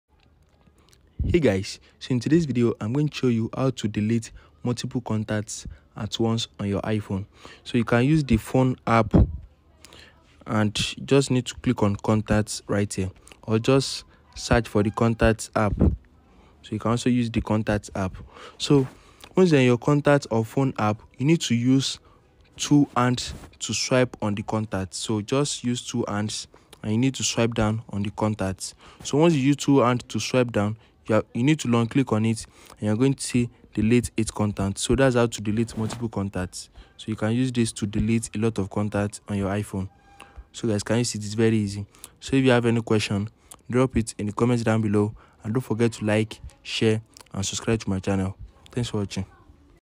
How to Delete Multiple Contacts At Once on iPhone
Tired of scrolling through your iPhone contacts one by one to delete unwanted contacts? In this video, we'll show you an easy and quick method to delete multiple contacts at once on your iPhone. With just a few simple steps, you can declutter your contacts list and free up valuable space on your iPhone. Our step-by-step guide will walk you through the process, so you can efficiently delete multiple contacts in just minutes. Say goodbye to the hassle of deleting contacts individually and watch our video now to learn how to delete multiple contacts at once on your iPhone!